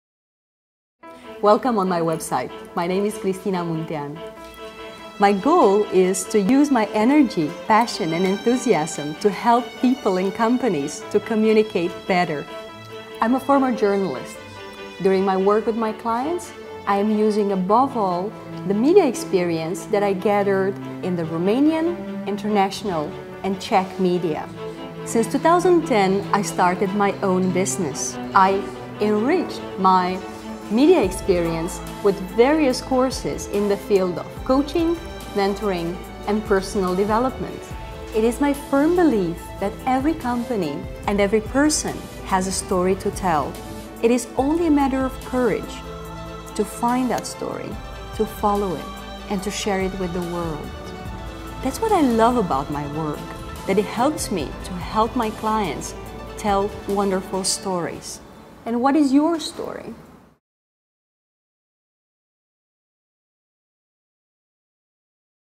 Presentation video Cristina Muntean
This is Cristina Muntean's business presentation video. Cristina supports leaders and companies to gain visibility and manage their reputation through media training, media coaching and mentoring, communications training and strategic communications and PR advisory. More about Cristina